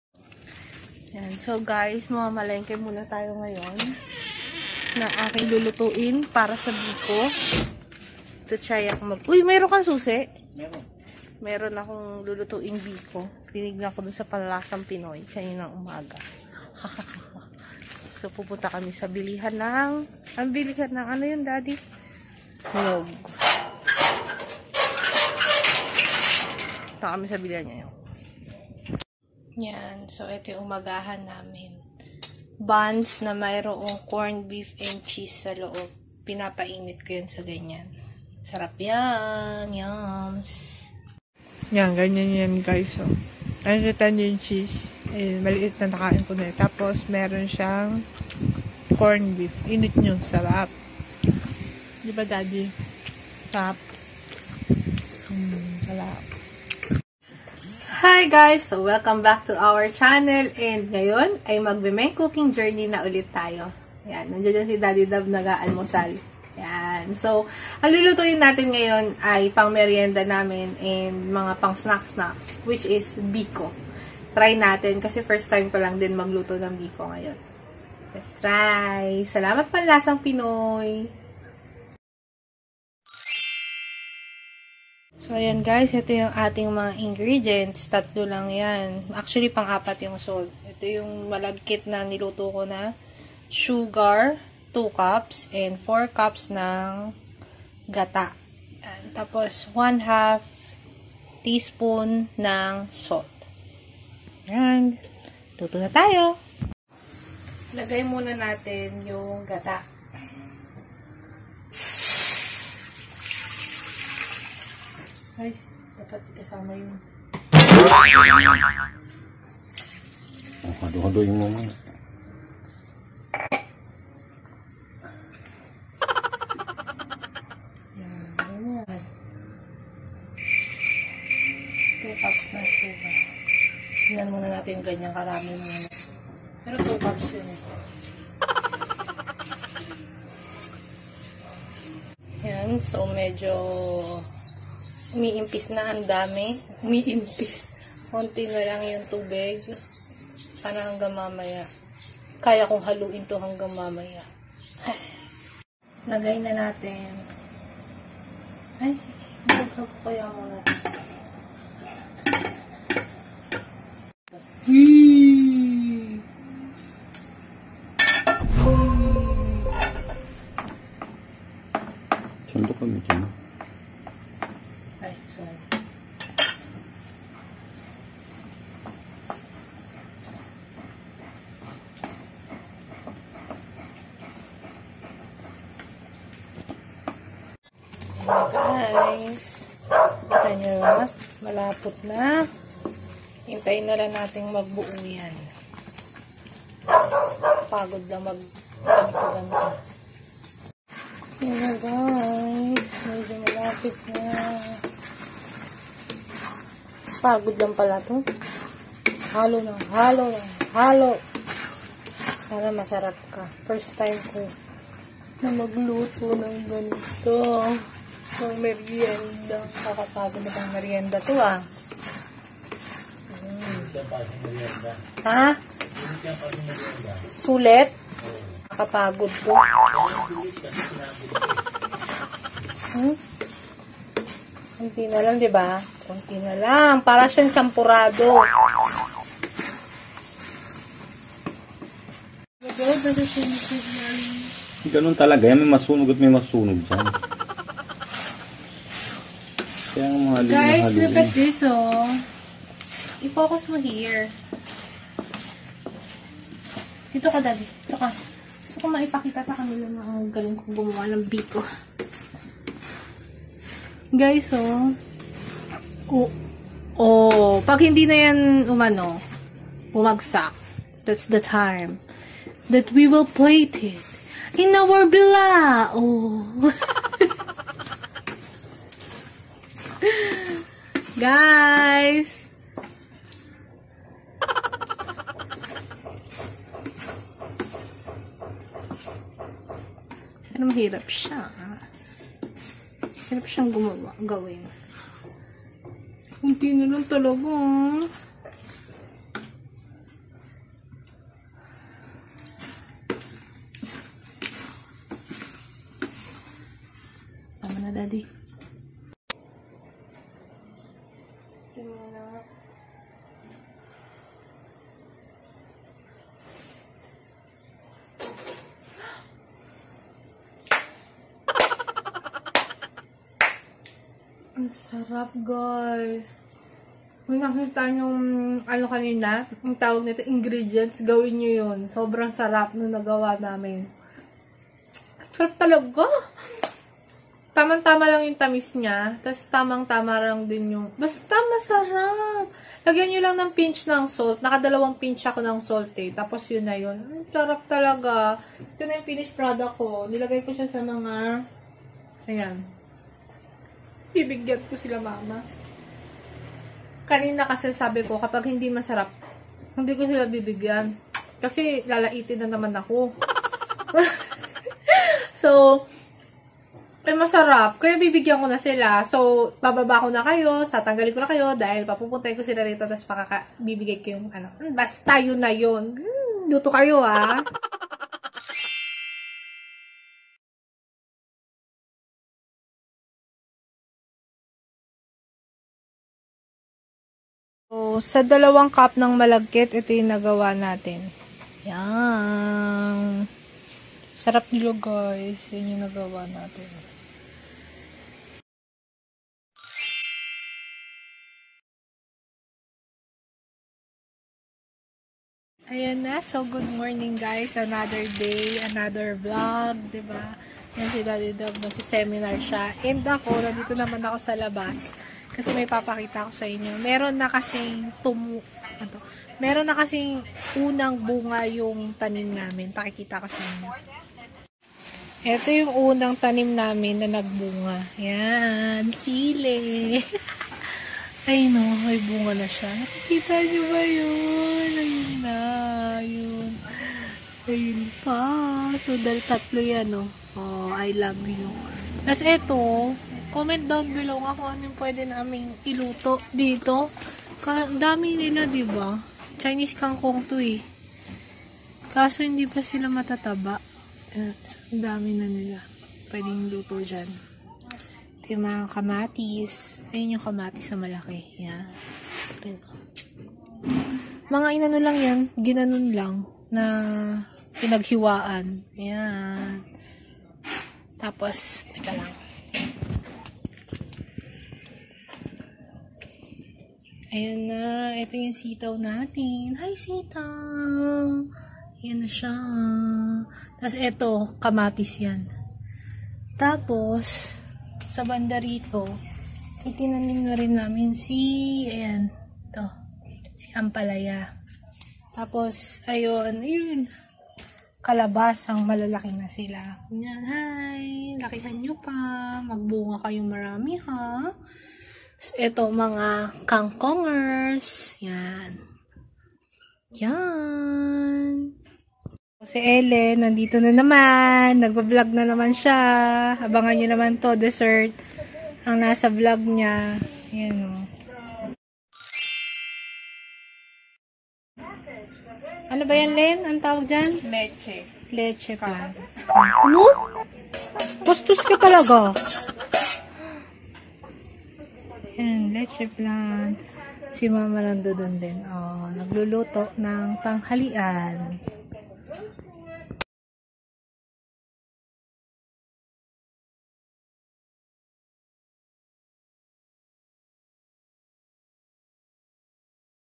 Simple Biko Recipe
Nakakapagod pero masarap na biko ang inyong magagawa. Ano pang hinihintay mo? Gawa ka na rin sa inyong bahay!